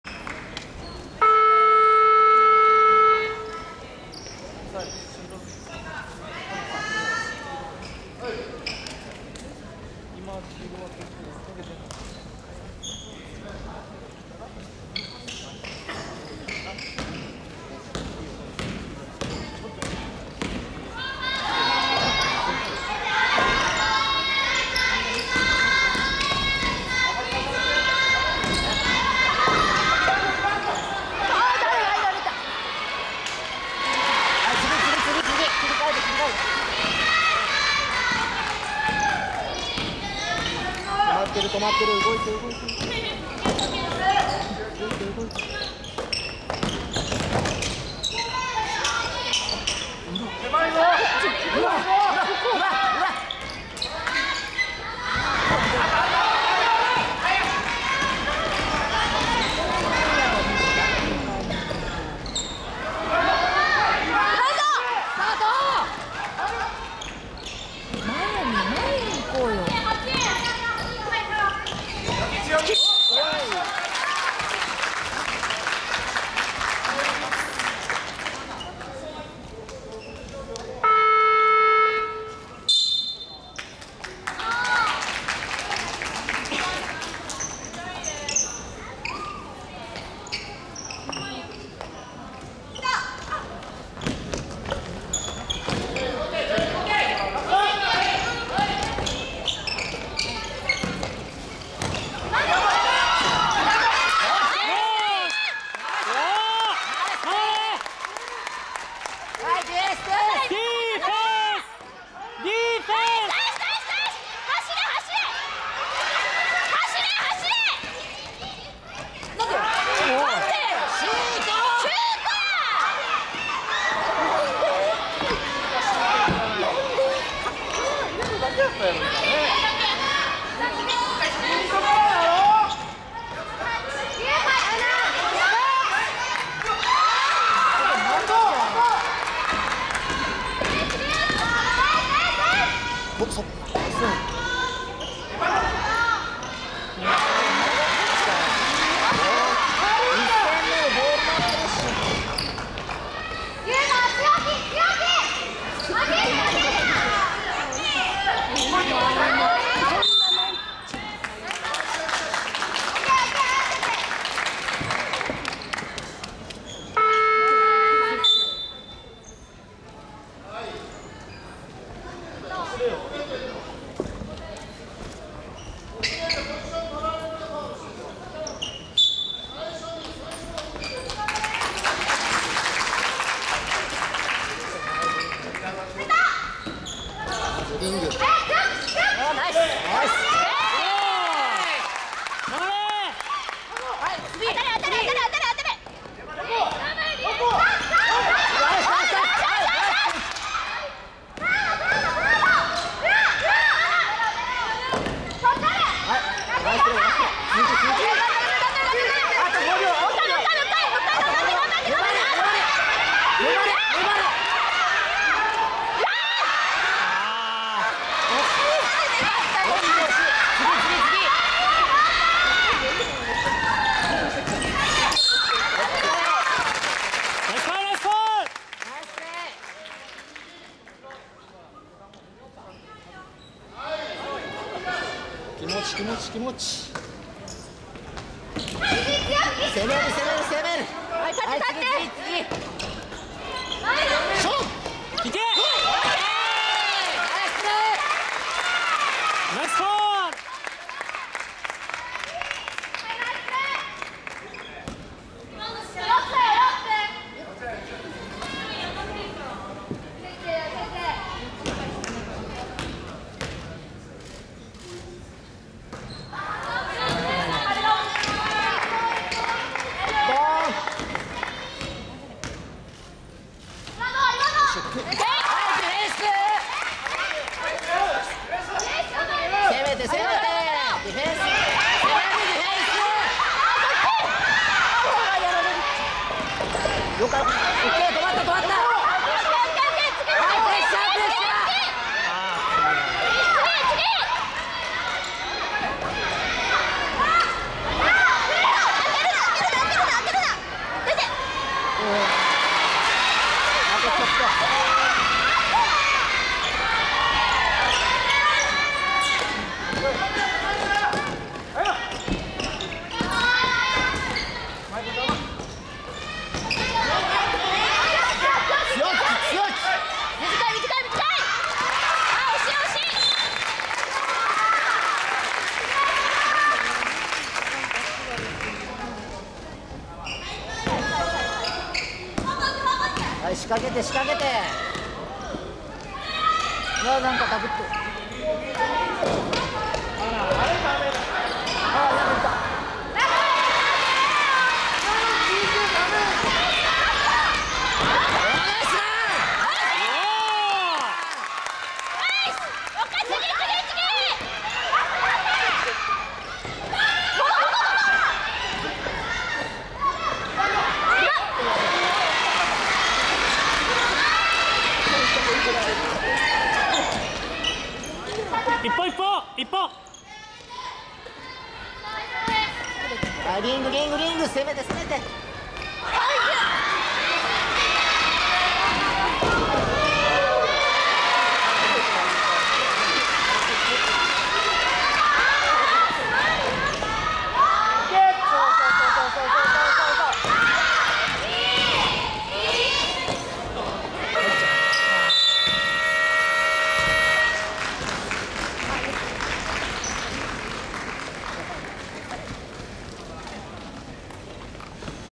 ウィンターカップ2014 鹿児島純心女子vs山形商業【6/ 全8本】
2014/12/23　東京体育館　D
ウィンターカップ2014
前半は、山形商業ペースで終わりますが
3ピリ、そして4ピリで純心が粘ります。
ロイ世代の、引退試合になりました。
お疲れ様でした！
ノーカット編集です。。。すみません。。。